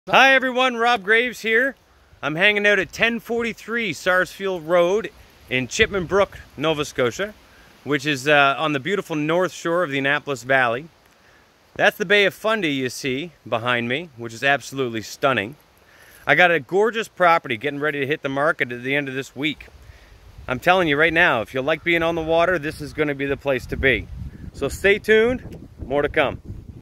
1043 Sarsfield Road, Chipman Corner
🔥SNEAK PEEK!🔥 Getting ready to hit the market! If you love the water your going to want to check this property out!